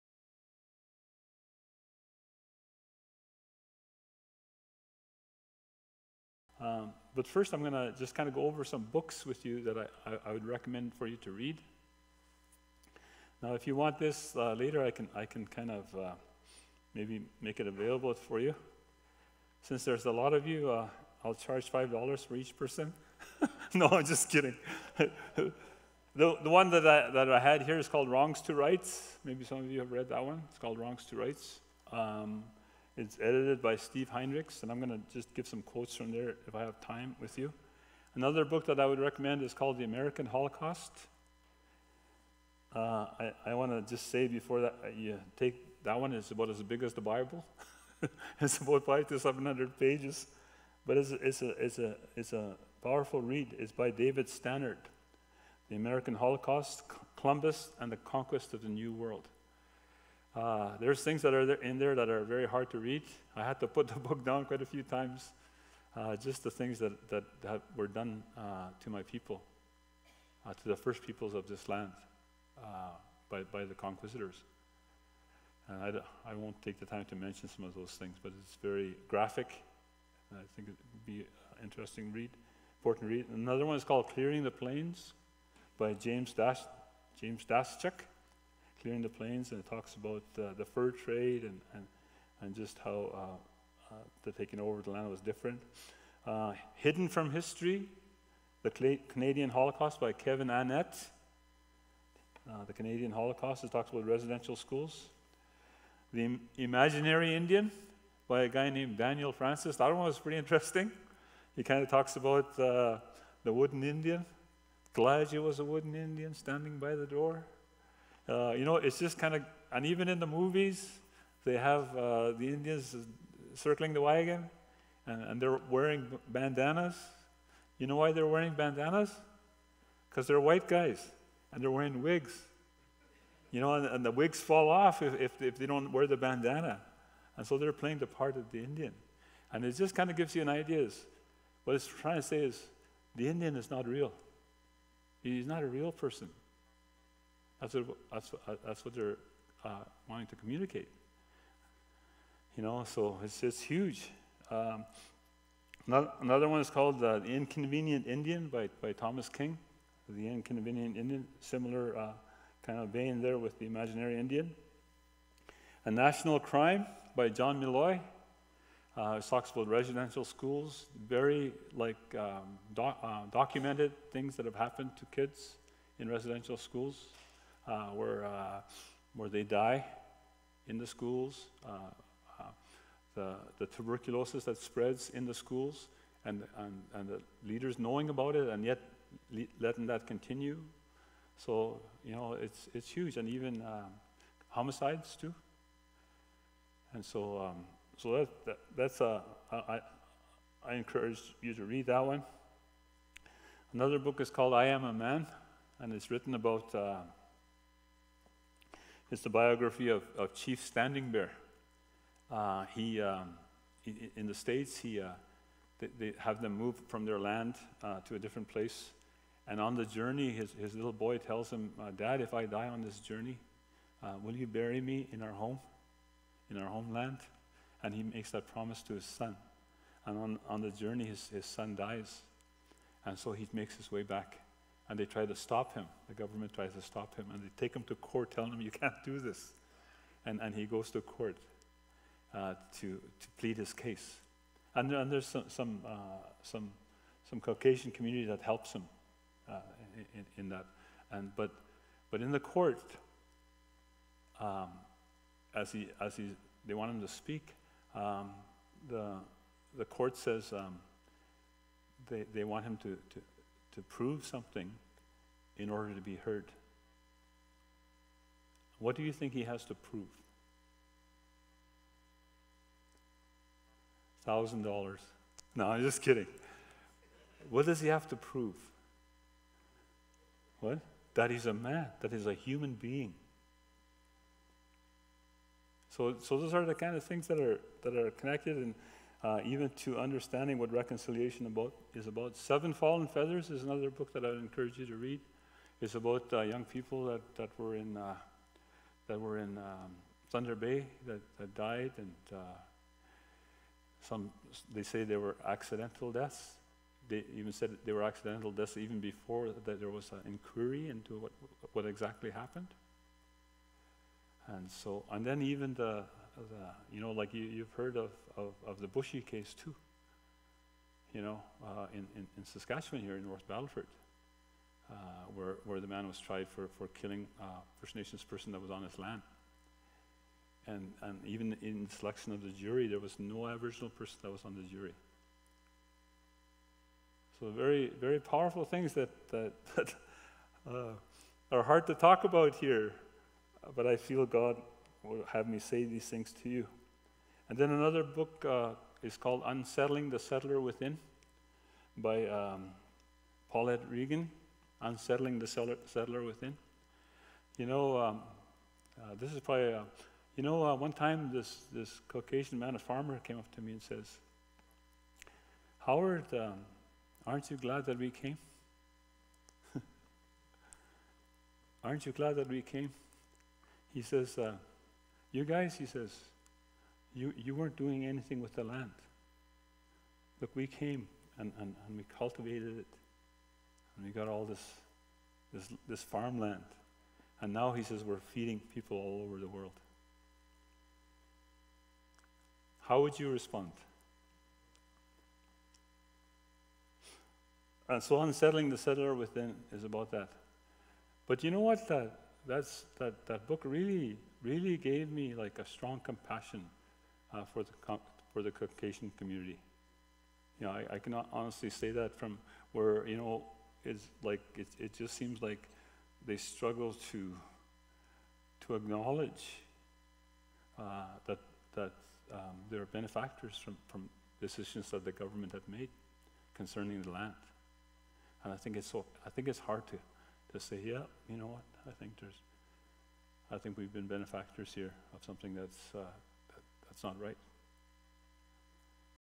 Reading List (Truth and Reconciliation)
Howard Jolly recommend these 10 books.

00:28 Wrongs to Rights: How Churches can Engage the United Nations Declaration on the Rights of Indigenous Peoples
00:40 American Holocaust: The Conquest of the New World by David Stannard
01:24 Clearing the Plains:  Disease, Politics of Starvation, and the Loss of Aboriginal Life by James Daschuk
01:36 Hidden from History: The Canadian Holocaust  by Kevin Annett
01:47 The Imaginary Indian: The Image of the Indian in Canadian Culture  by Daniel Francis
02:36 The Inconvenient Indian: by Thomas King
02:46 National Crime: The Canadian Government and the Residential School System by John S. Milloy
03:24 "I Am a Man": Chief Standing Bear's Journey for Justice by Joe Starita
04:53 Seven Falling Feathers by Tanya Talaga
05:48 Unsettling the Settler Within: Indian Residential Schools, Truth Telling, and Reconciliation in Canada by Paulette Regan